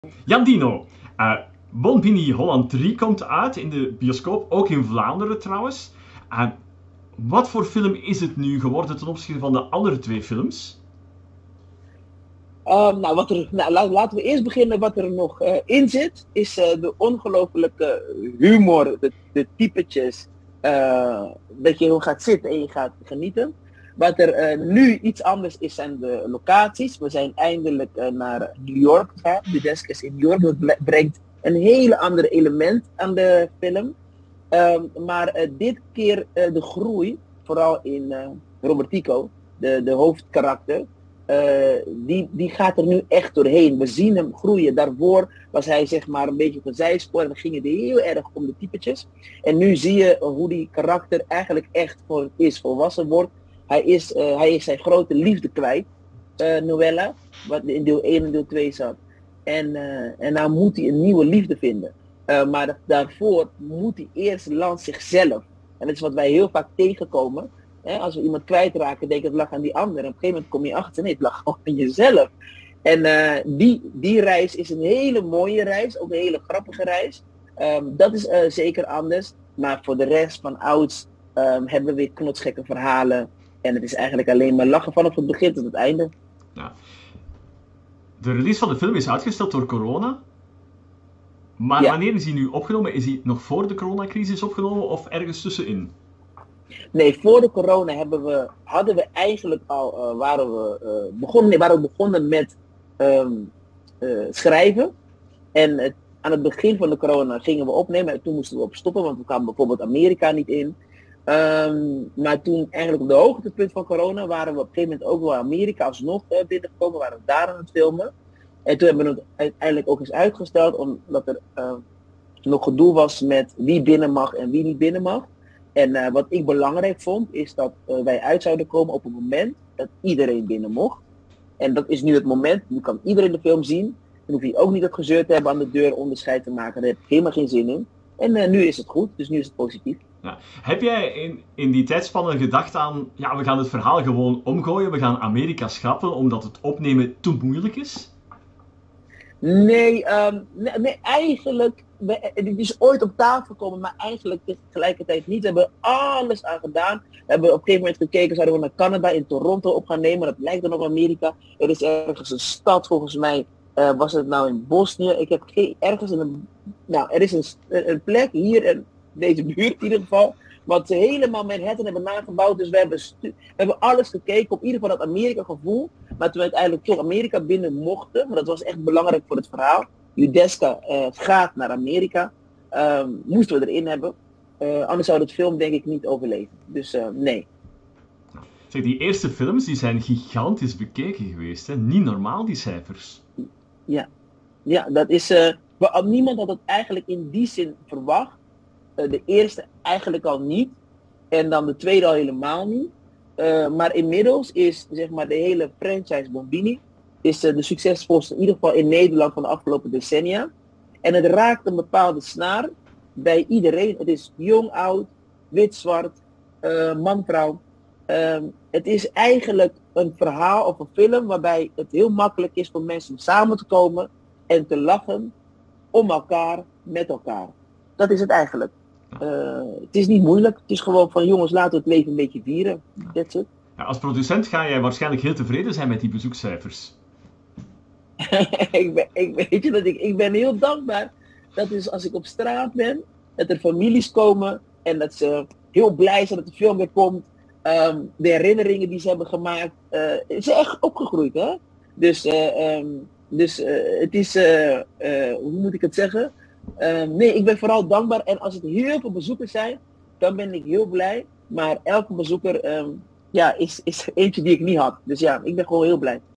Jandino Asporaat maakt derde film Bon Bini Holland 3
De nieuwe komische speelfilm van de succesvolle BON BINI reeks krijgt wederom een internationaal podium.  De film verscheen in Nederland, Aruba, Bonaire, Curaçao én in België. Jandino Asporaat is een held, want hij maakte de film niet alleen, hij speelt ook nog eens de hoofdrol in dit dolkomisch avontuur.